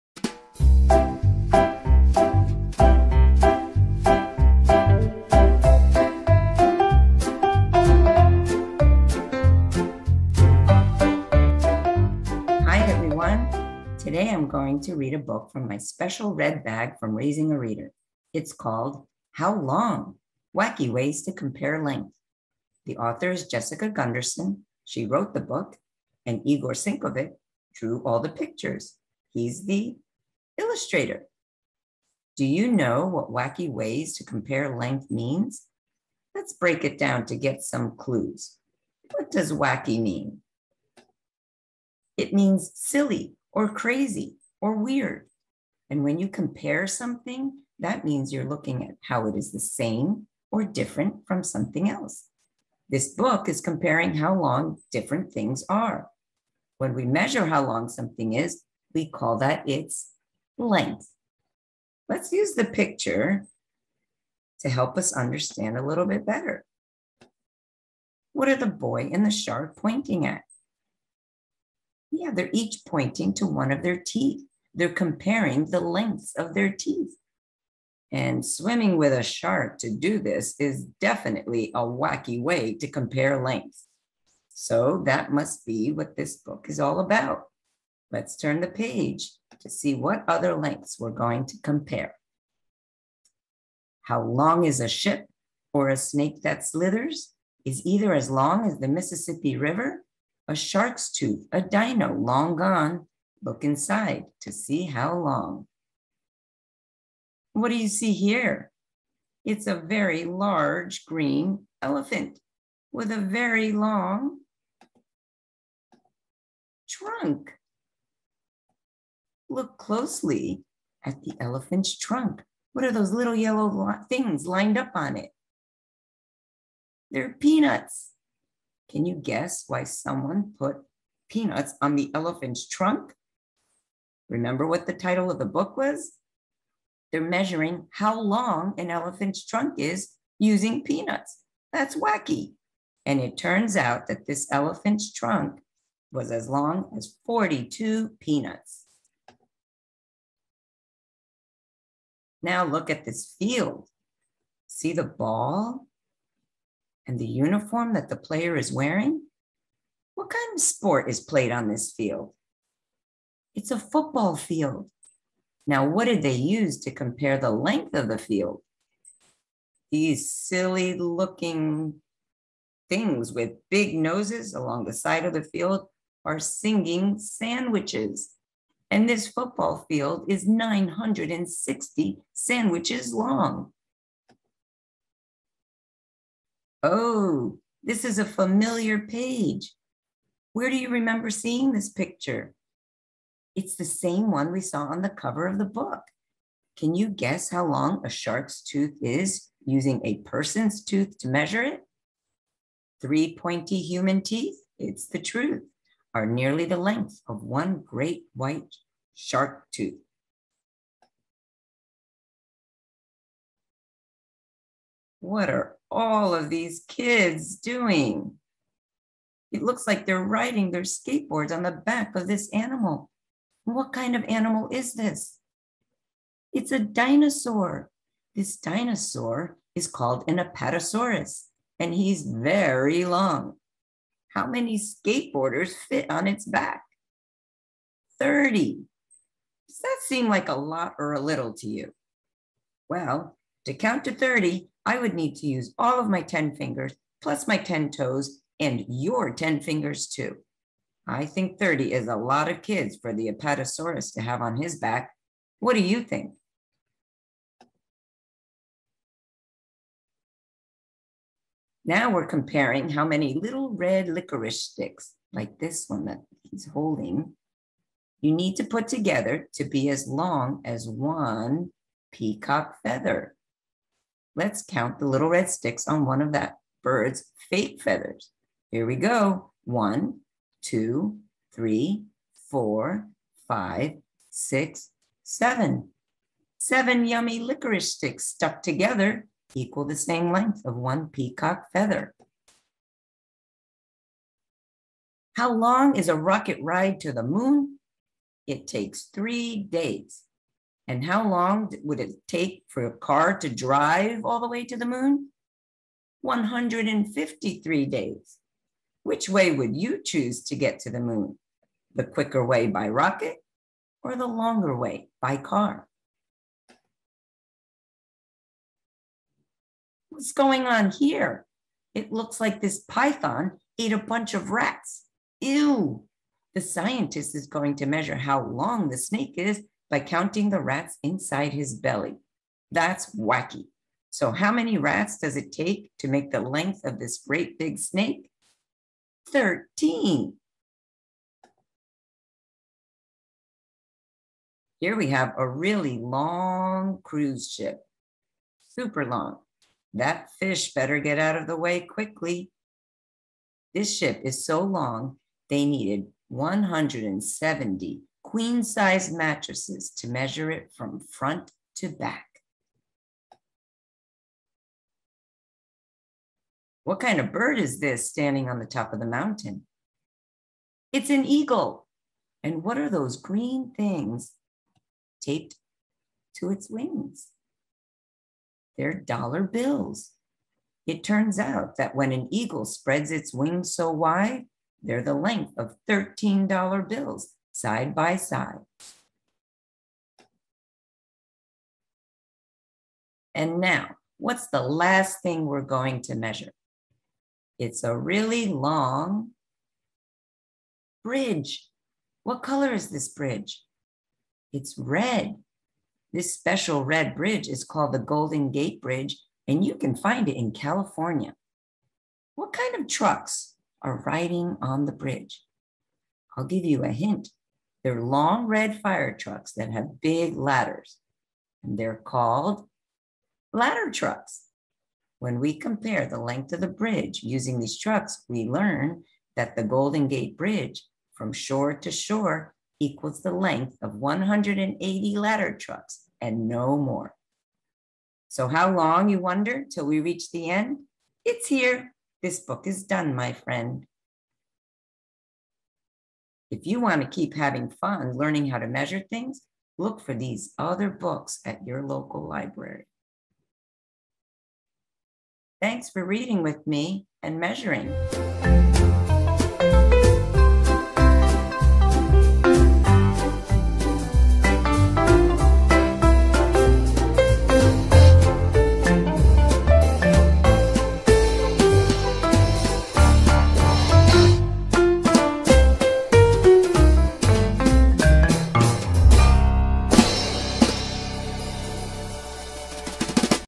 How Long Wacky Ways to Compare Length (STEAM)
Book: How Long?
Author: Jessica Gunderson
Illustrator: Igor Sinkovec
Publishing House: Philomel Books
Video Language: English
Ages: 4-7
Speaker: Catalina Rojas
Produced by RAR-MA for Raising A Reader

Like to see more activities and types of events?